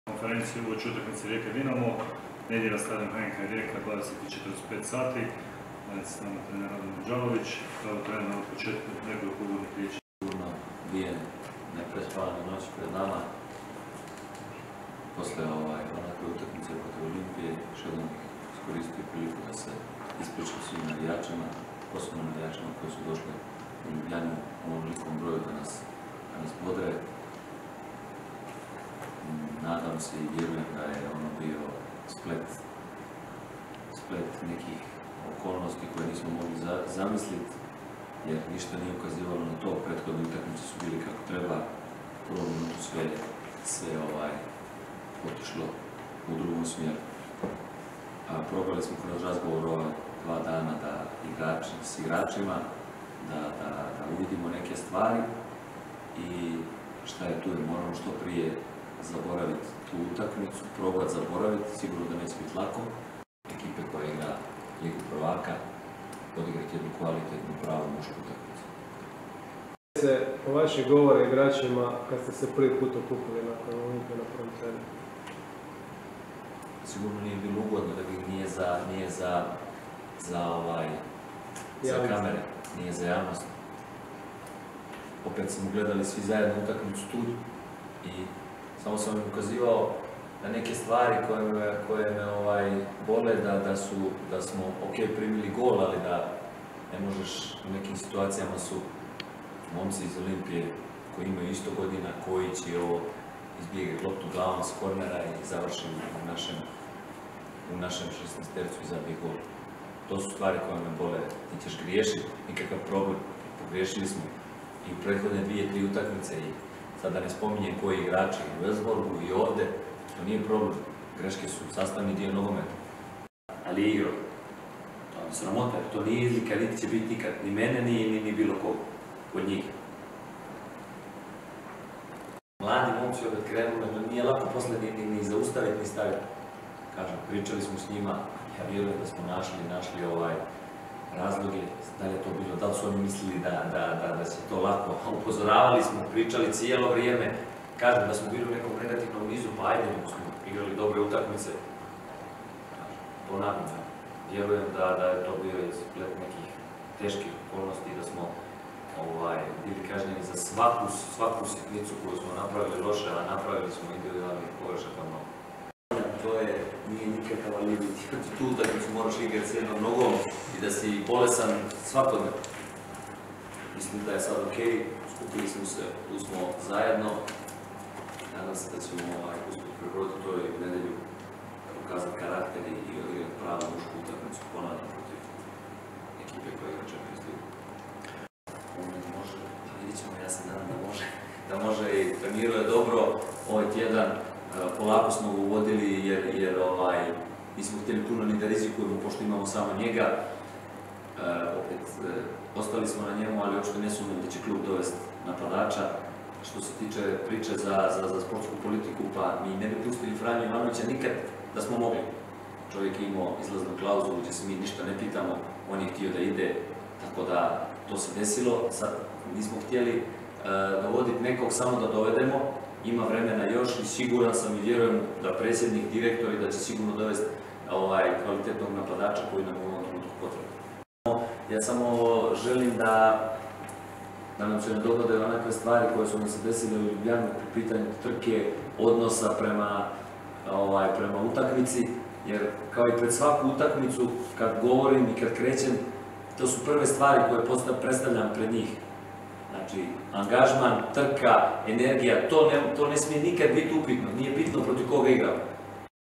Radomir Đalović prokomentirao stanje u ekipi i najavio utakmicu protiv Dinama
Trener HNK Rijeke Radomir Đalović na konferenciji za medije najavio utakmicu protiv Dinama i prokomentirao stanje u ekipi nakon utakmice u Ljubljani.